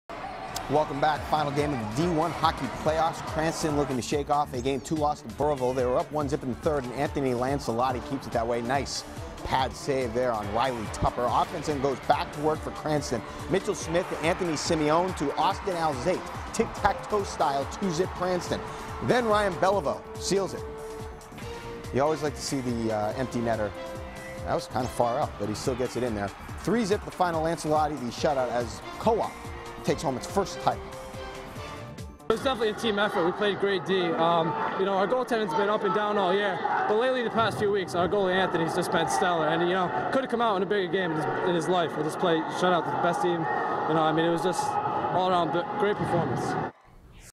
Cranston Co-op Secures Div. I Title
Cranston shut out Burrillville 3-0 in the final game of the Division I championship series.